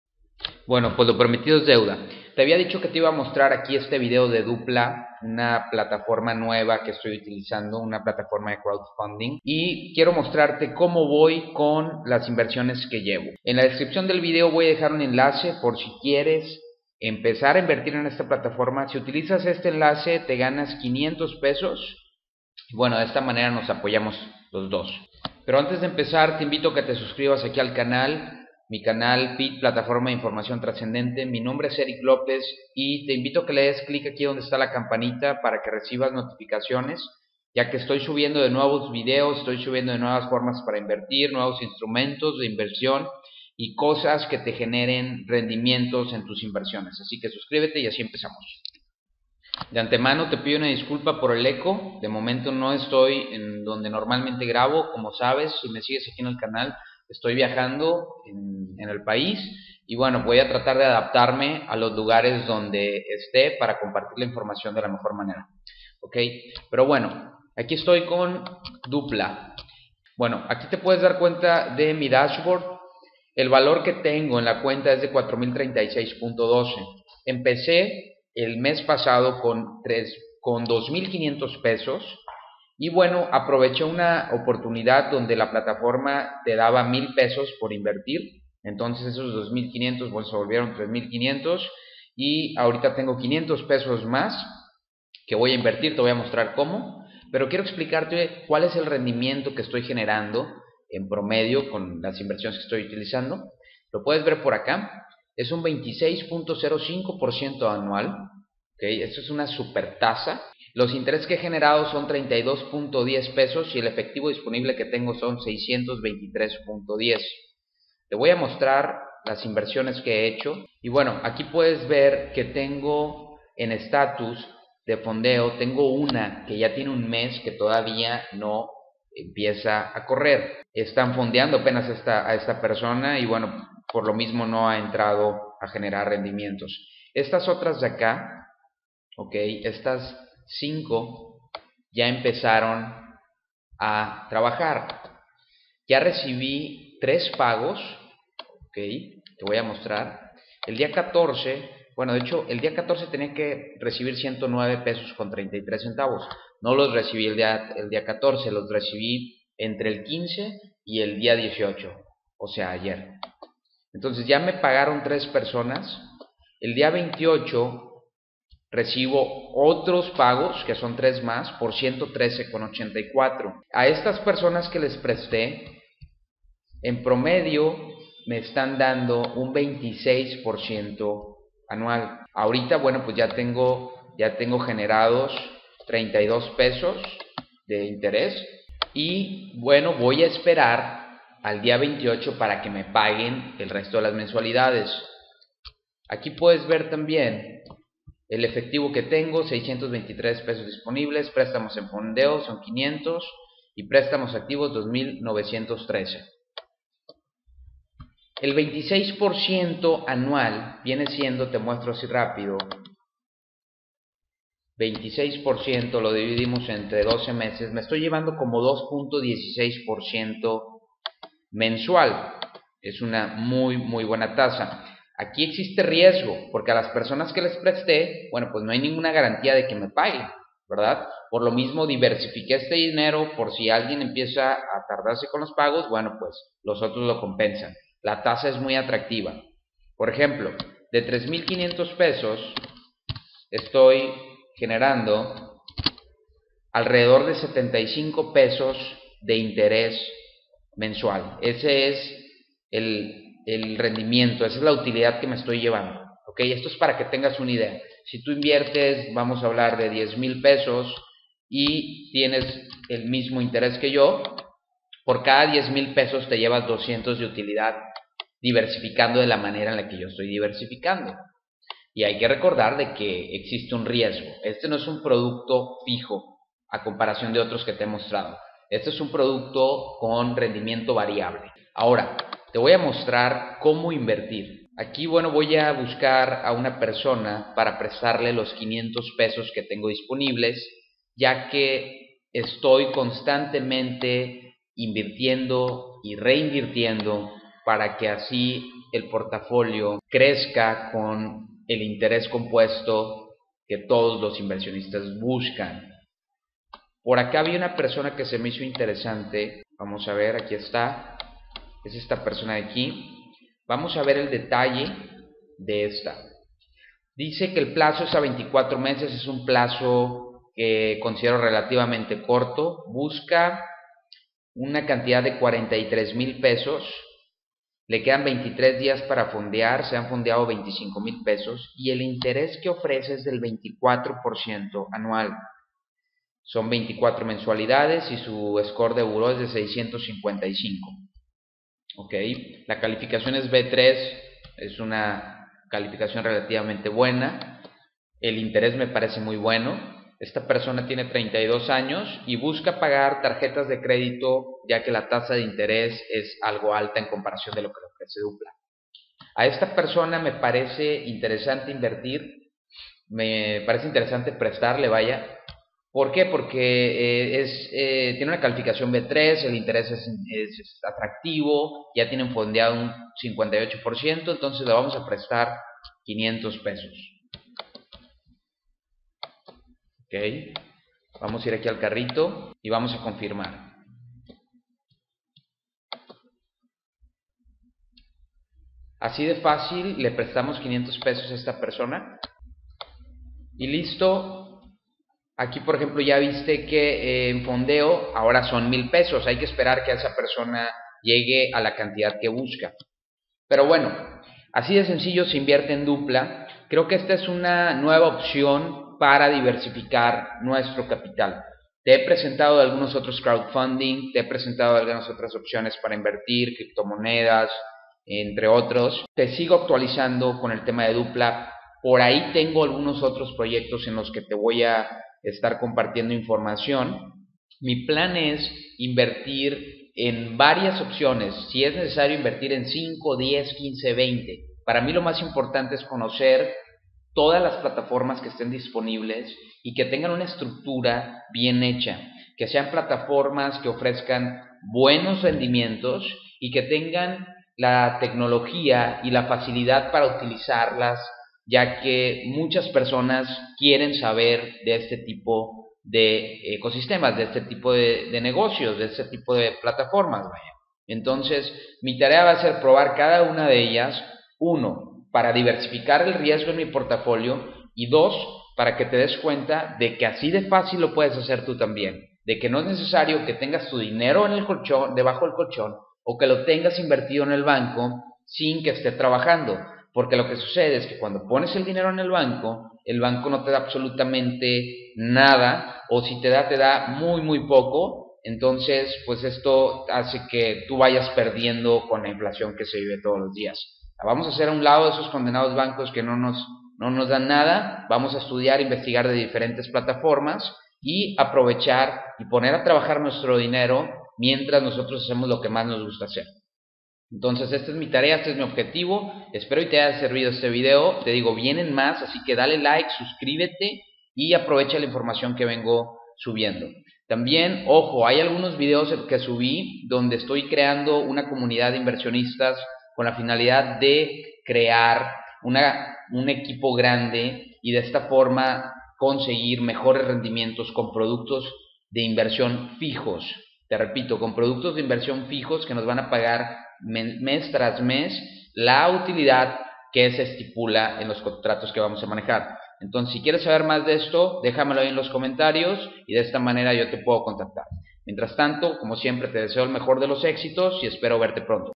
Como ganar Dinero Invirtiendo en Doopla
Te comparto como sigo ganando dinero con Doopla, plataforma Crowdfunding Peer to Peer lending osea de prestamos de persona a persona. Te comparto el enlace por si quieres utilizarla.

Quieres aprender a invertir en Bitcoins, te comparto mi libro:

COMPRA DE BITCOINS MEXICO